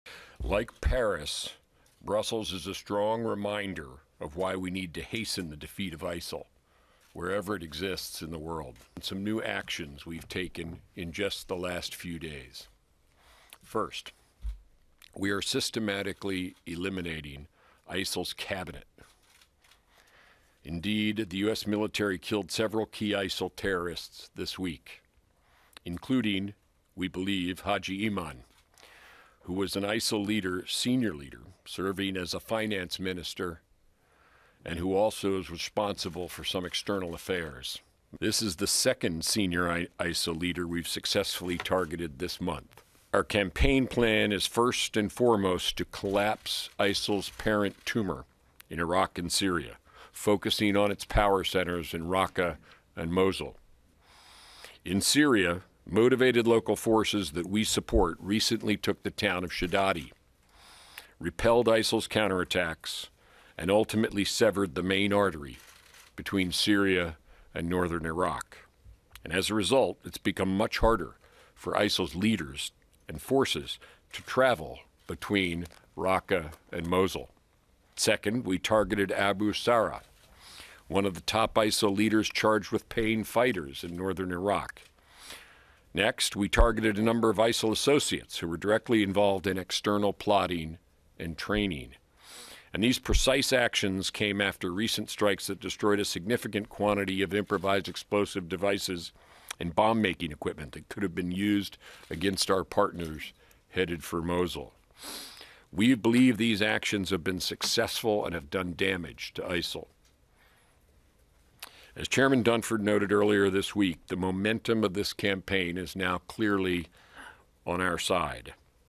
ISIS No. 2 Killed in Raid
U.S. Defense Secretary Ash Cater said the key leader served as finance minister.